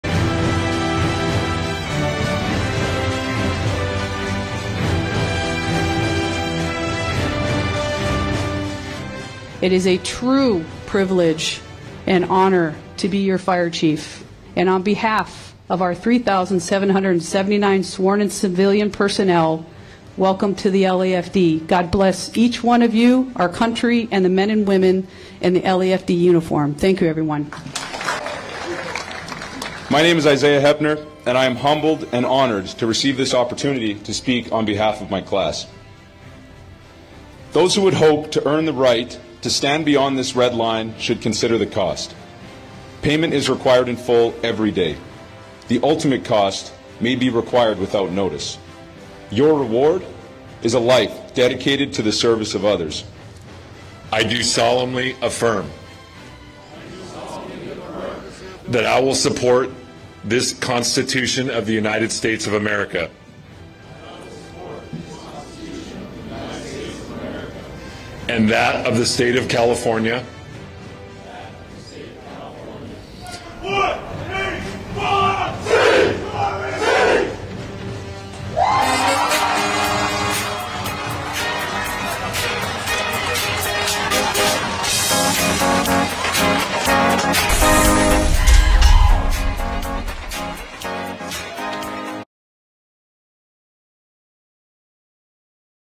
LAFD Welcomes Graduates of Recruit Training Academy Class 2022-4
PANORAMA CITY- The men and women of @LosAngelesFireDepartment's Recruit Training Academy Class 2022-4 celebrated their class graduation on June 15, 2023, as they became proud probationary members of the LAFD.

Friends and family of the graduates were joined by City of Los Angeles Fire Chief Kristin Crowley and civic and community leaders, in congratulating and welcoming the fifty-one newest members of the Department in a tradition filled ceremony.

[Video by John McCoy]